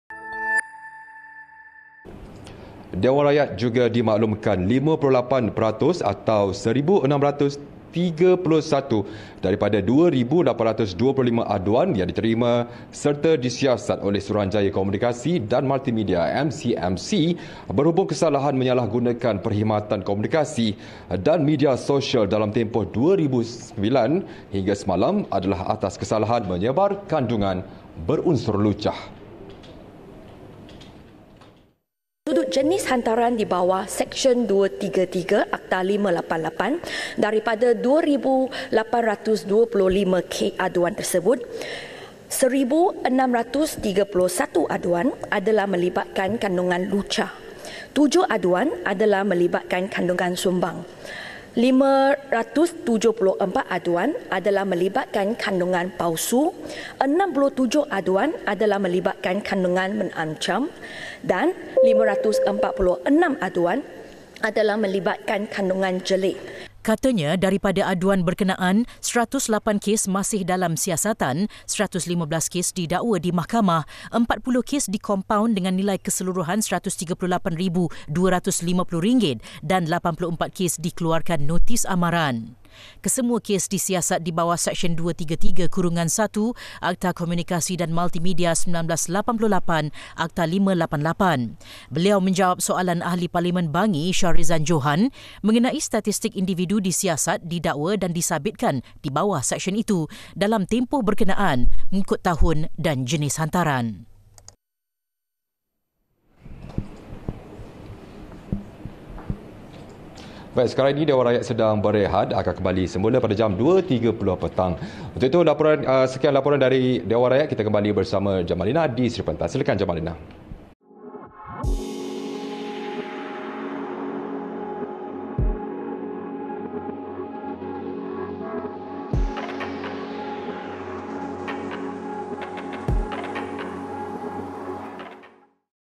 DEWAN RAKYAT | Kongsi Kandungan Lucah Catat Aduan Tertinggi Diterima MCMC
Dewan Rakyat dimaklum 58% atau 1,631 daripada 2,825 aduan yang diterima serta disiasat oleh Suruhanjaya Komunikasi dan Multimedia (MCMC)